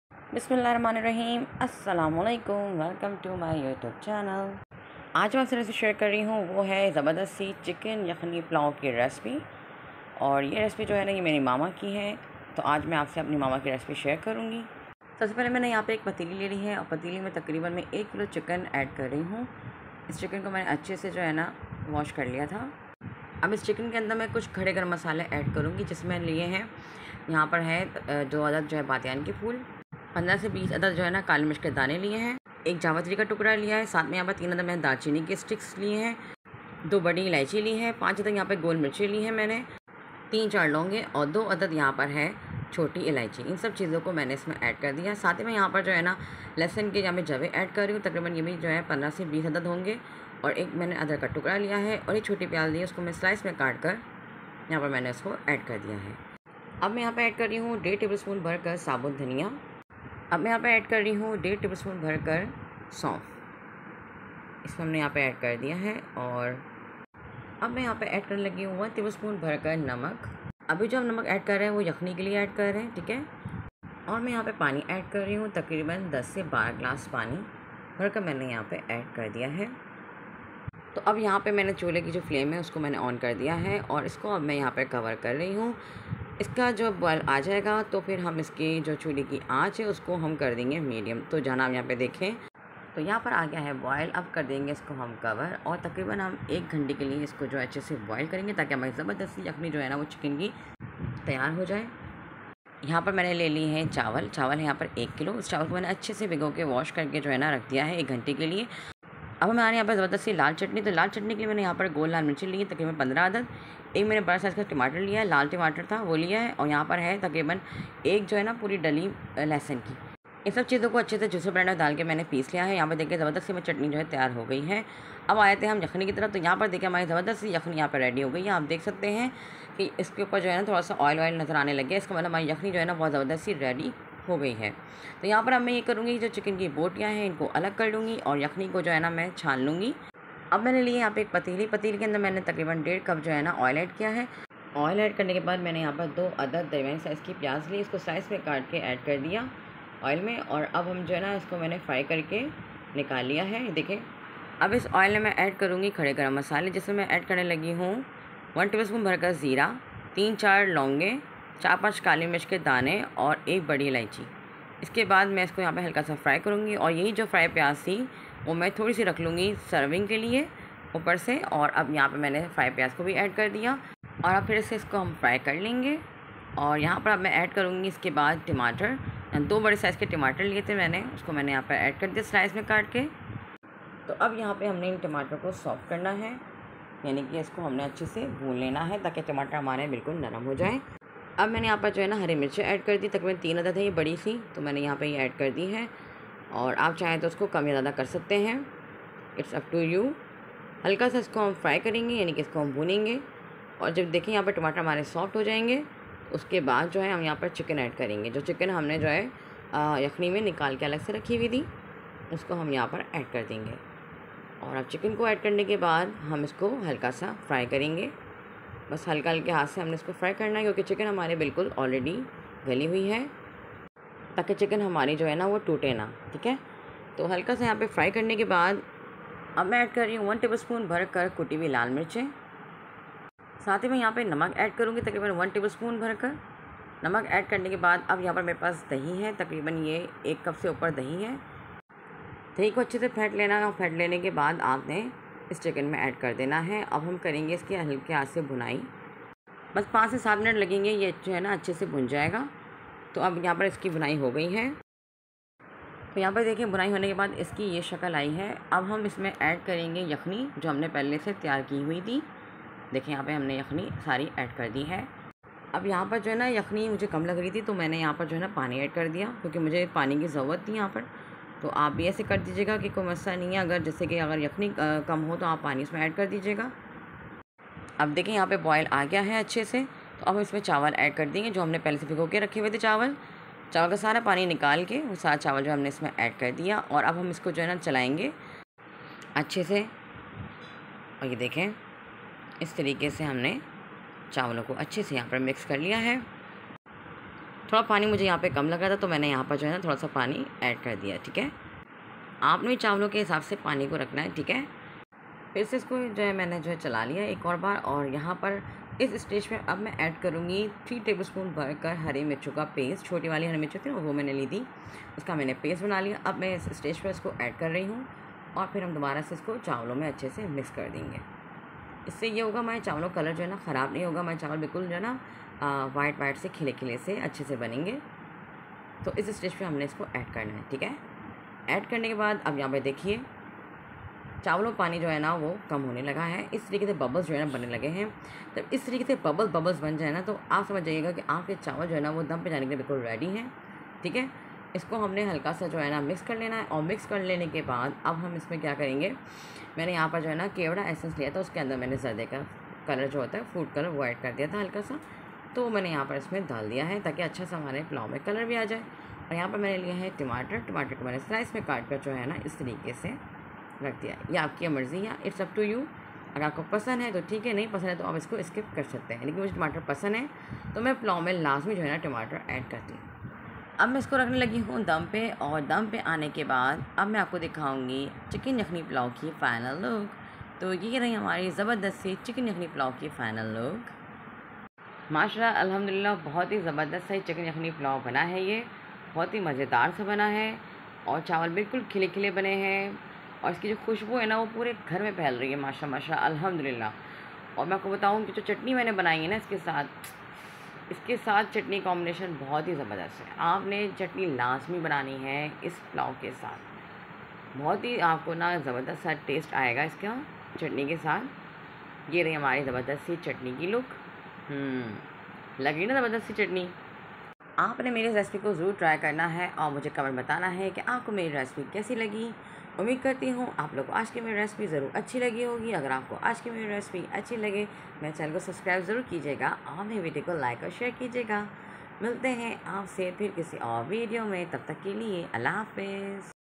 How To Make Chicken Yakhni Pulao By Easy Cooking With Faiza - Chicken Pulao Recipe
Chicken Pulao Recipe.A step by step complete recipe that How To Make Chicken Yakhni Pulao by Easy Cooking With Faiza.A Pakistani recipe Chicken Pulao or Indian Recipe of Chicken Yakhni Pulao.
yakhni pulao
pulao recipe
chicken yakhni pulao recipe
chicken pulao
 how to make chicken yakhni pulao